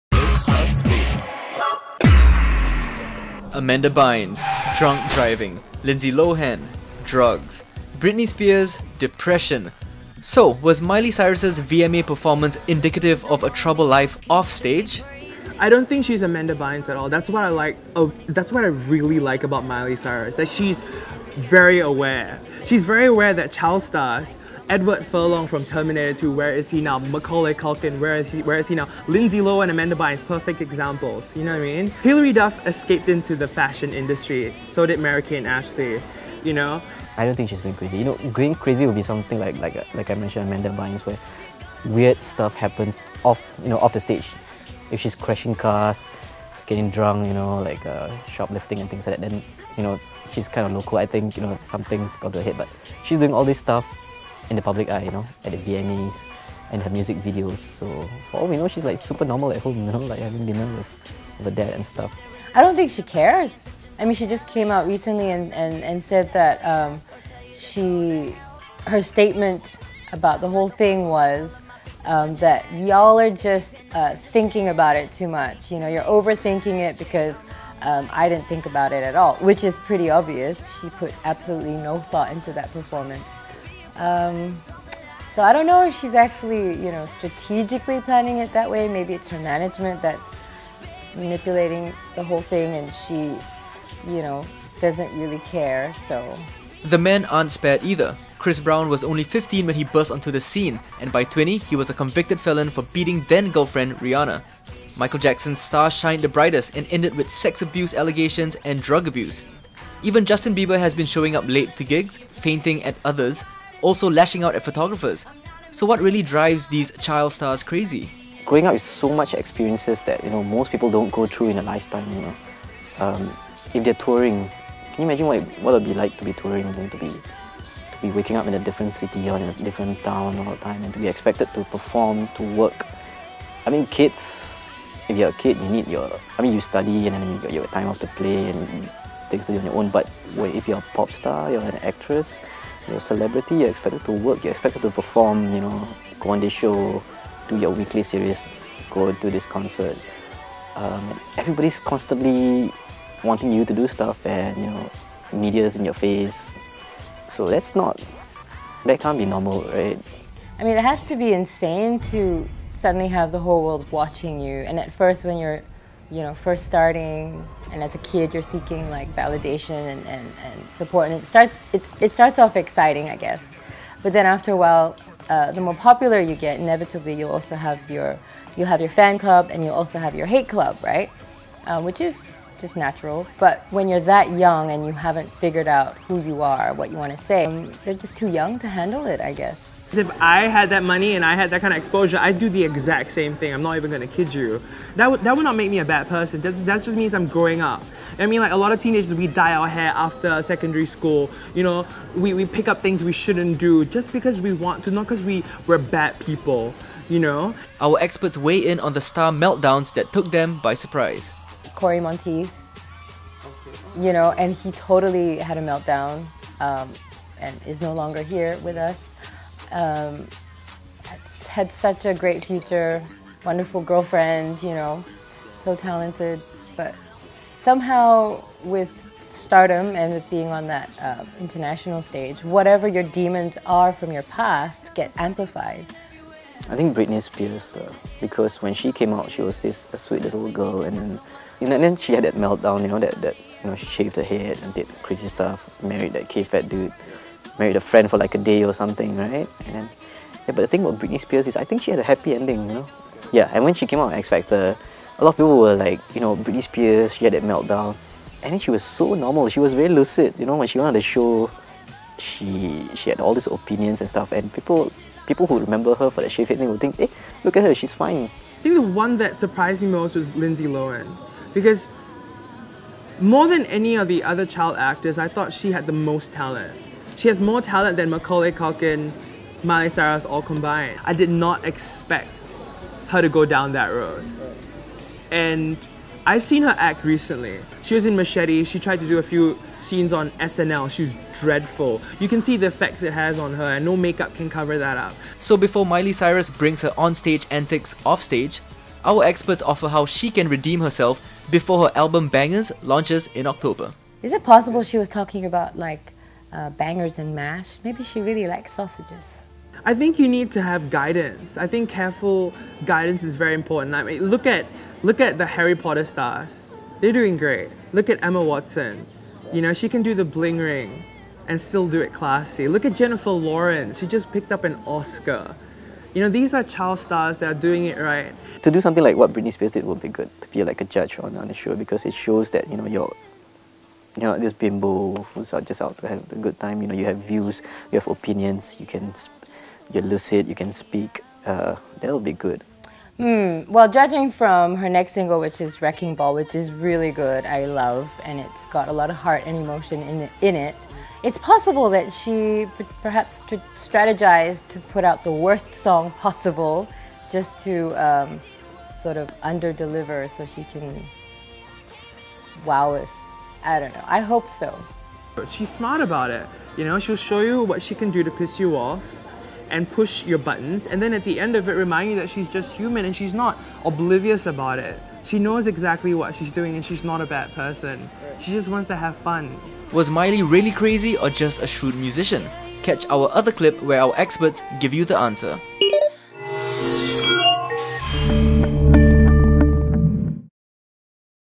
Child stars gone wild
Michael Jackson. Britney Spears. Lindsay Lohan. Is Miley Cyrus the next child star to go off the rails? Our music experts decipher what makes them tick and how Miley can redeem herself.